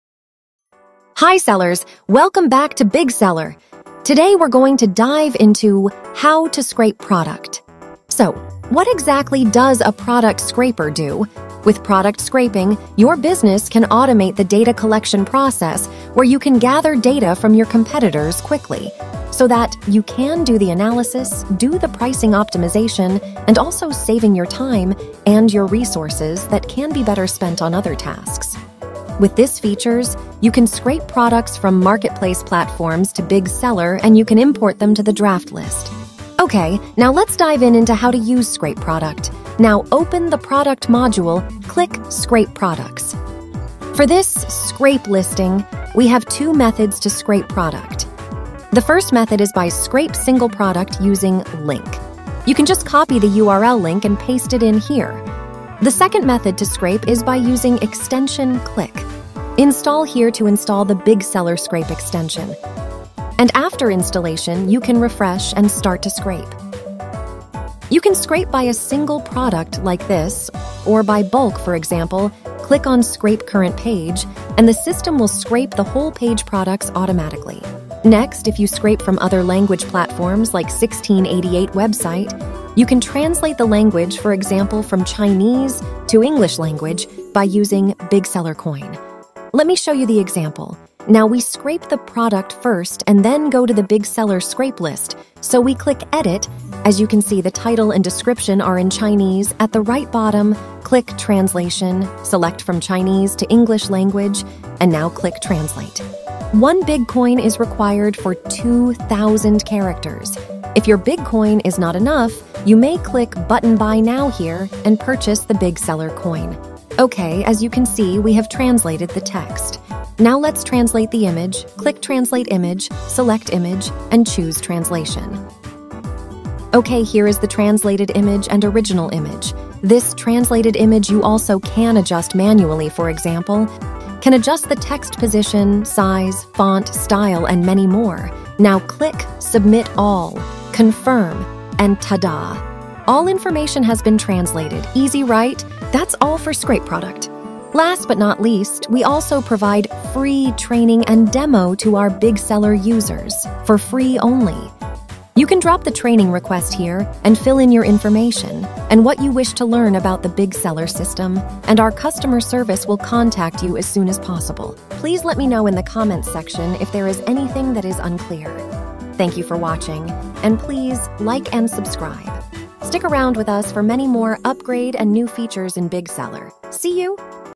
How to Scrape Product. Stop Copy-Pasting Product Listings!
Welcome to this series of videos on How to Scrape Product in BigSeller. With Bigseller Product Scraper, you can easily copy products from other sellers to your own accounts. But to ensure the safety of your accounts, do modify the product info before uploading those products to the shops.

✒️ Main features:
0:00 Introduction
0:29 Marketplace Platform that can connect with BigSeller
0:35 Scrape by Link
0:56 Scrape by Extension
1:24 Translate Scrape Language
2:27 Request Free Training here!~
2:46 Join our Group Support